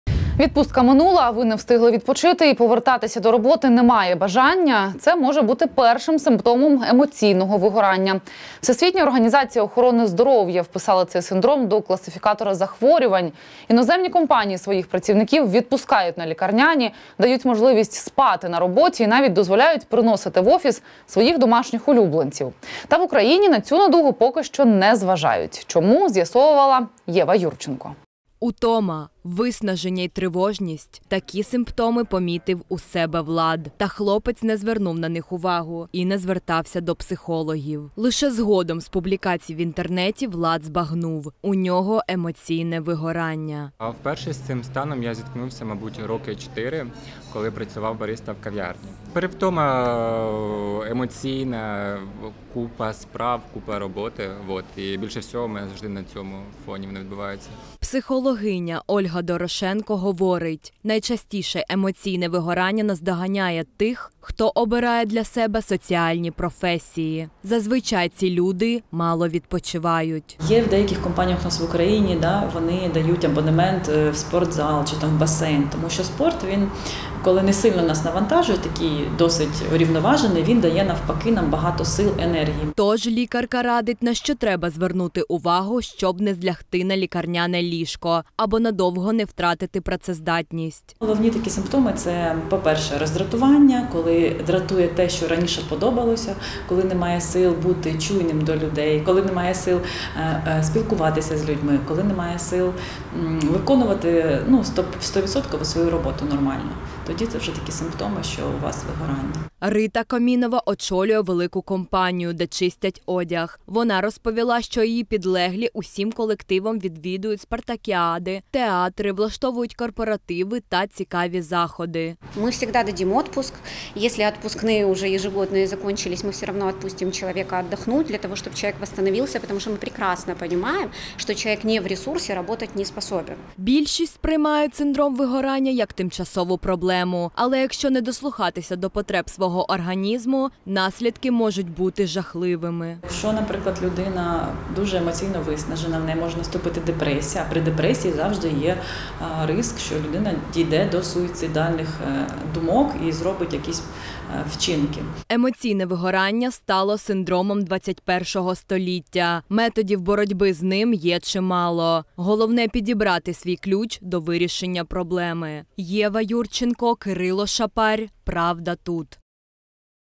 Що таке емоційне вигорання та як з ним боротися
Як розпізнати емоційне вигорання, чим воно загрожує та чи дають з цим новим
 діагнозом оплачуваний лікарняний в Україні